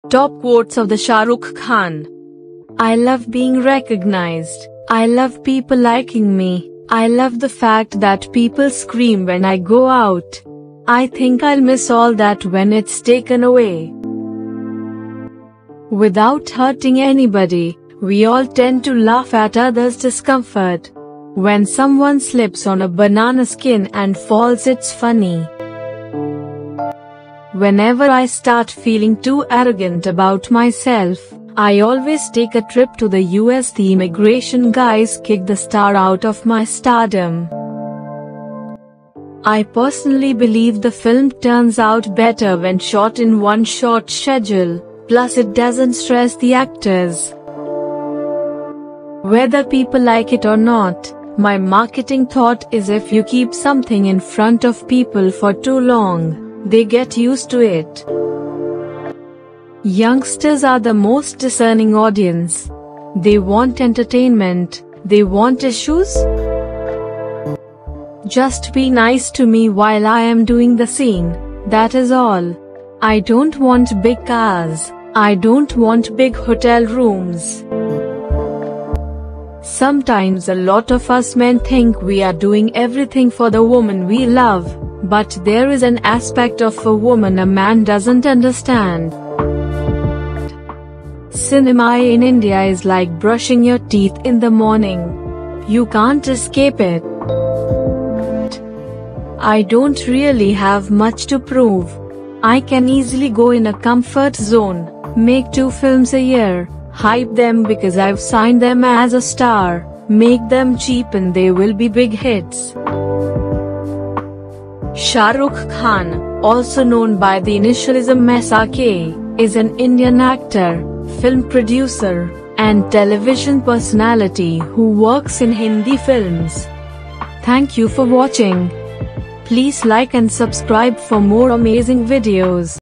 Top Inspirational Quotes of the ShahRukh Khan
- I love being recognized, I love people liking me, I love the fact that people scream when I go out. I think I'll miss all that when it's taken away.
- Without hurting anybody, we all tend to laugh at others' discomfort. When someone slips on a banana skin and falls it's funny.
- Whenever I start feeling too arrogant about myself, I always take a trip to the U.S. The immigration guys kick the star out of my stardom.
- I personally believe the film turns out better when shot in one short schedule, plus it doesn't stress the actors.
- Whether people like it or not, my marketing thought is if you keep something in front of people for too long, they get used to it.
- Youngsters are the most discerning audience. They want entertainment, they want issues.
- Just be nice to me while I am doing the scene; that is all. I don't want big cars, I don't want big hotel rooms.
- Sometimes a lot of us men think we are doing everything for the woman we love, but there is an aspect of a woman a man doesn't understand.
- Cinema in India is like brushing your teeth in the morning. You can't escape it.
- I don't really have much to prove. I can easily go in a comfort zone, make two films a year, hype them because I've signed them as a star, make them cheap and they will be big hits.
- Shah Rukh Khan, also known by the initialism SRK, is an Indian actor, film producer, and television personality who works in Hindi films.